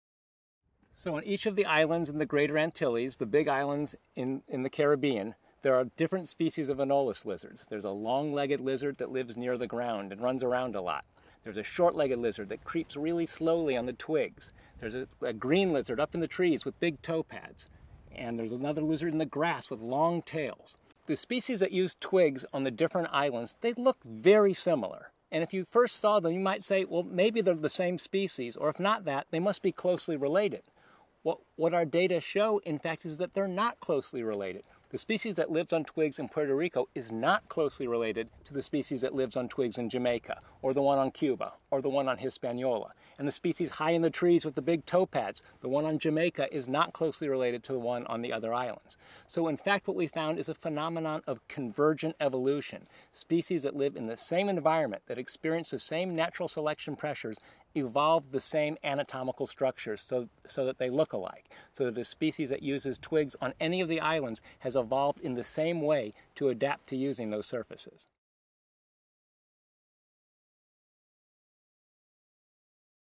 Convergent Evolution of Lizards — HHMI BioInteractive Video
Species of anole lizards with similar body types and adaptations have evolved again and again on each of the four larger Caribbean islands: Puerto Rico, Jamaica, Cuba, and Hispaniola. Hear Harvard University biologist Jonathan Losos describe this compelling example of convergent evolution.

This classroom-ready video is perfect for science students learning about evolution and the effects of changing habitats.